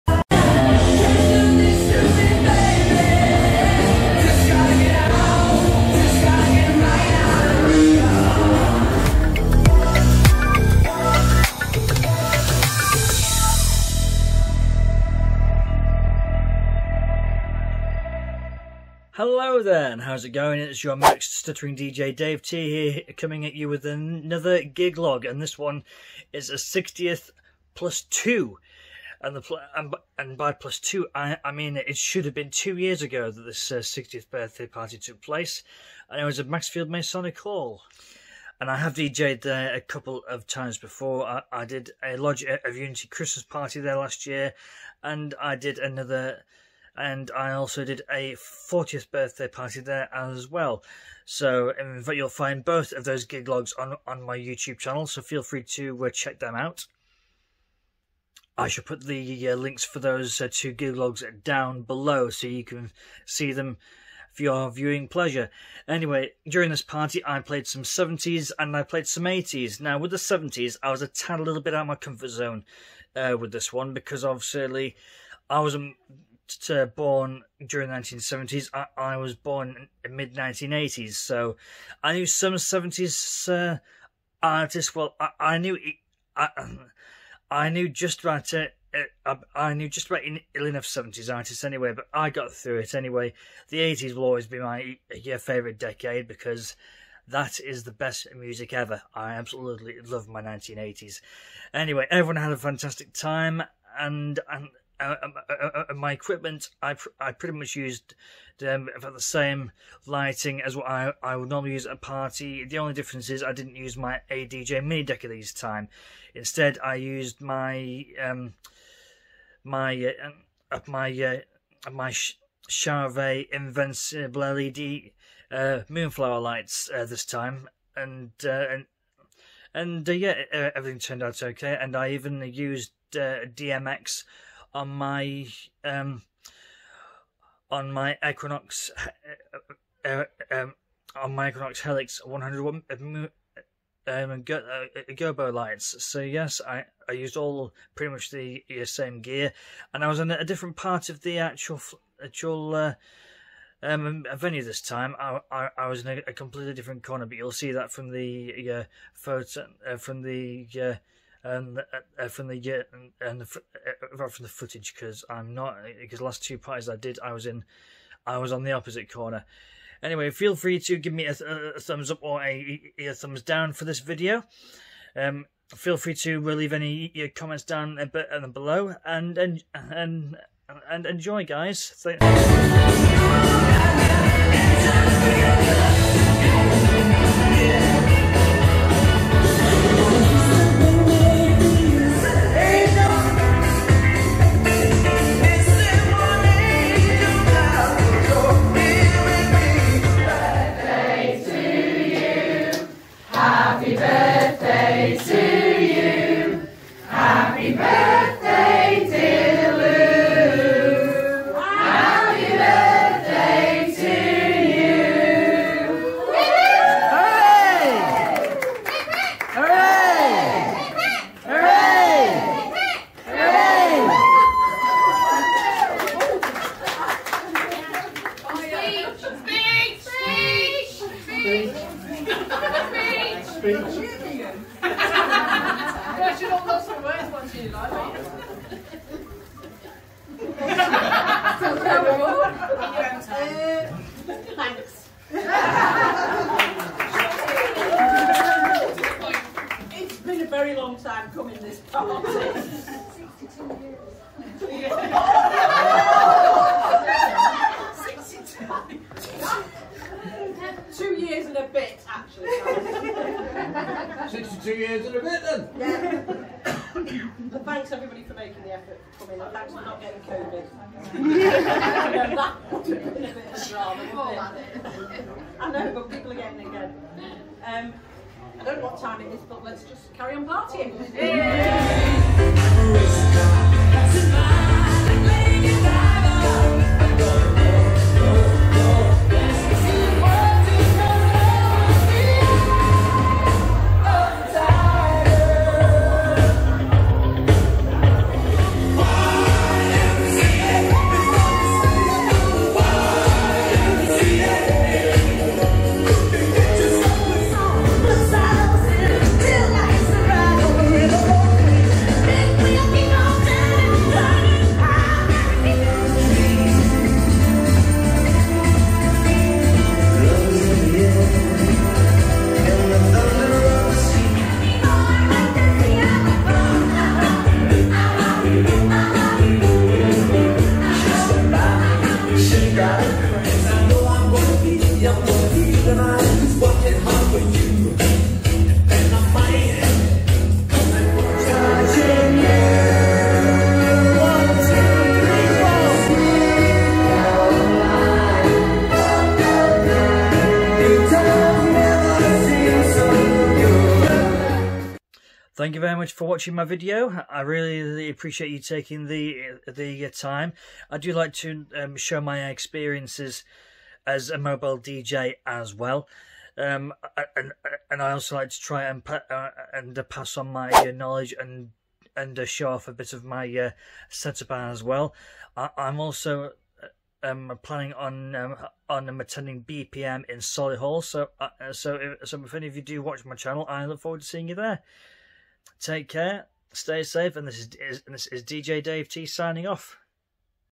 Delayed 60th Party
I had the pleasure of doing a delayed 60th birthday party for Louise Carbutt ,  her friends & family at Macclesfield Masonic Hall.  Obviously me being a local DJ I knew some people there.

I played a mixture of 70's & 80's music on the night. The room decoration & part was organised by her daughter & daughter inlaw who were both lovely & down to earth.

Loading my equipment was easy in & out. My equipment was as follows:
Equinox DJ booth with Twin Bar overhead Gantry.

Lighting
This product is no longer made.
Chauvet Dj Invincbled Moonflower affect.

Now my 1st Room filler
Equinox Helix 100 Watt gobo affect light

Sound System Equipment
Software is Virtual DJ 2020

RCF Evoke J8 1400 Watt Speaker Arrays.

I also used a HP Laptop with 1 TB Samsung Portable Harddrive.

I used my Samsung Galaxy S21 to film the video.

Thank you for taking the time to view my gig log.